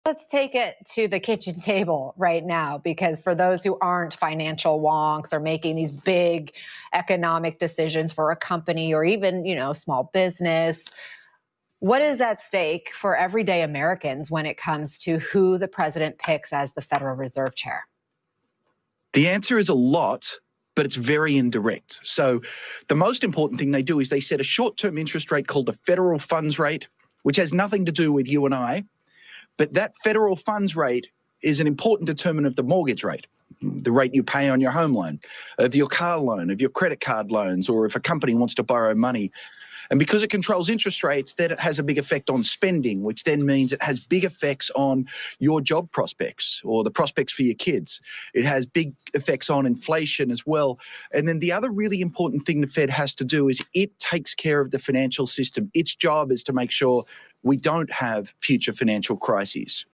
Federal Reserve Chair: Your Money & Job at Stake!
The Federal Reserve Chair sets a short-term interest rate. This impacts mortgage rates, car loans, credit cards, and business borrowing. It affects spending, job prospects, inflation and the stability of the financial system.